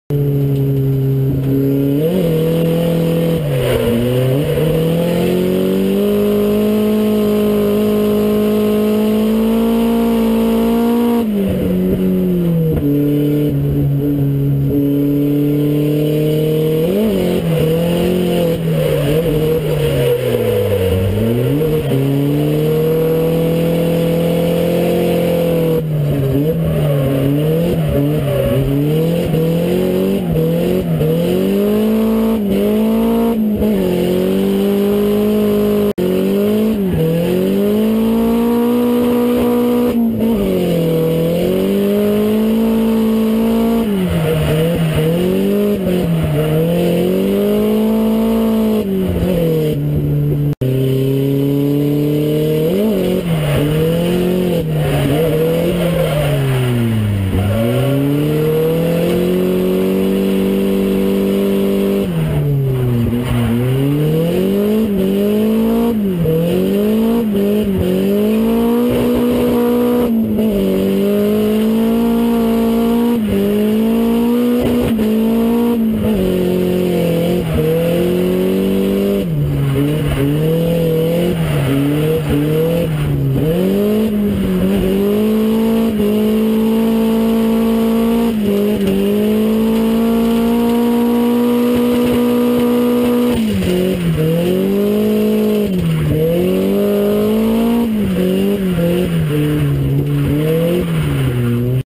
lydden hill bhp show 2014, Drift demo incar footage SR86.
Just out there having fun killing tyres!